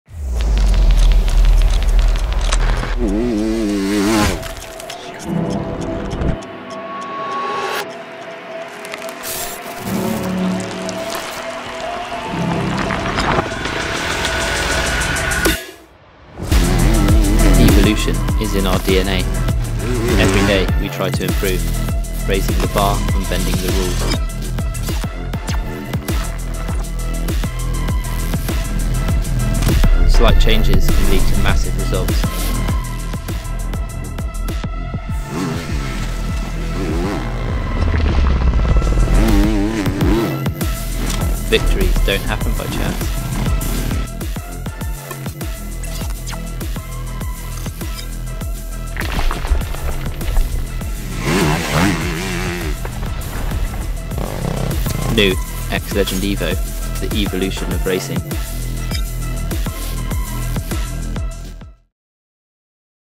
Eleveit X Legend EVO
Discover Eleveit X Legend EVO boots, the choice of the 9 times World Enduro Champion Steve Holcombe, designed to raise the bar of lightness, safety and performance. X Legend EVO is the evolution of racing.